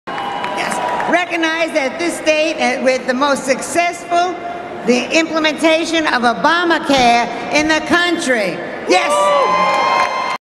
Connecticut's Democratic Delegation Brags About It's "Financially Unstable" Obamacare Co-Op
"About 40,000 people will lose their health insurance in the coming months as a result of a state evaluation that has deemed the financial health of Connecticut’s nonprofit health care co-op unstable."